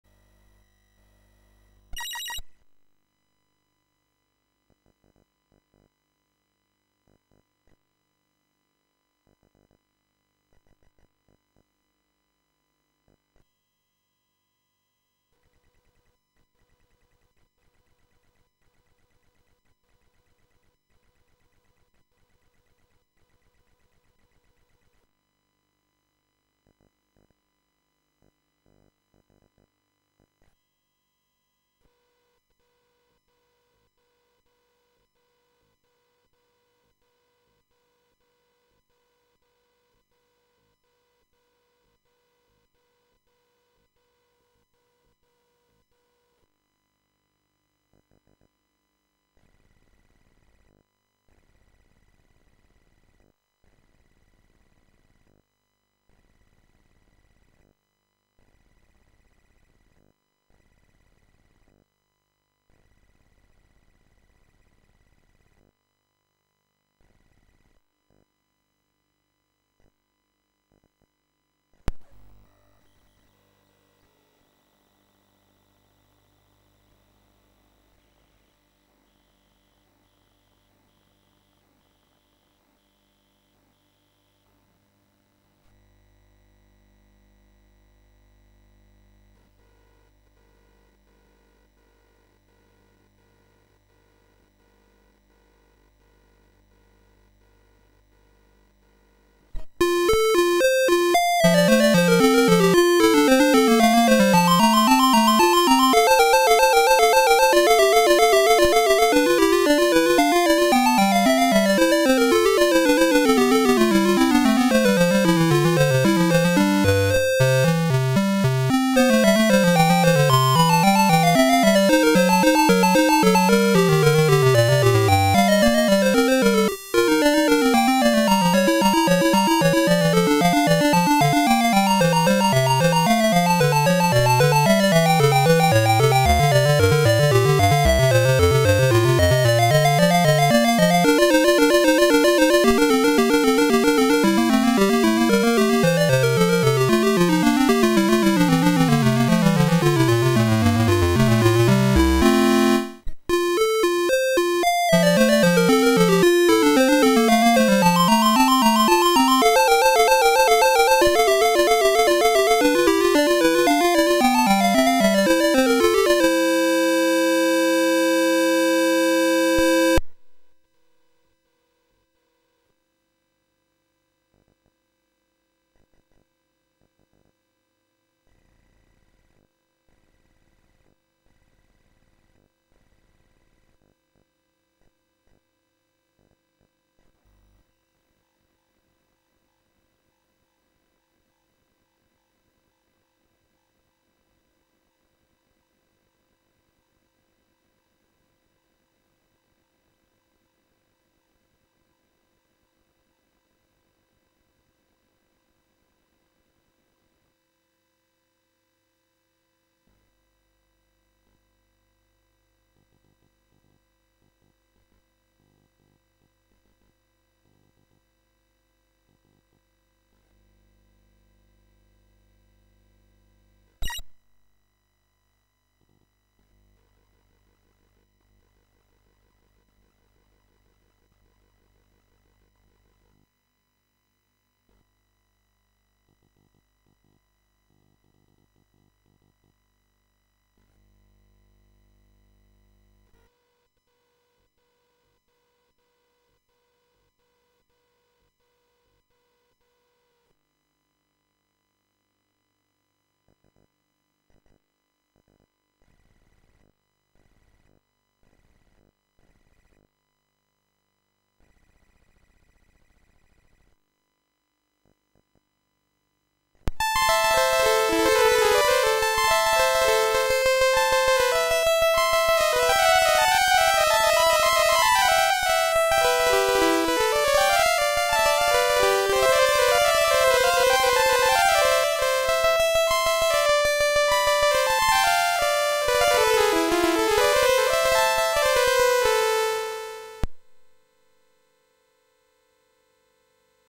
C128: Comal80 cartridge for the C128, running C64 COMAL demo disk
Won an eBay auction for a Comal80 cartridge for the C128.

This video shows my C128 running some demo programs from the demodisk for the C64 version of COMAL80.

If somebody knows where to find a C128 version of the COMAL80 demo disk, I would very much like to hear.

(Recorded from a real Commodore 128D with the real COMAL 80 cartridge using RGBtoHDMI for 80 column mode and Framemeister xRGB Mini for 40 column mode)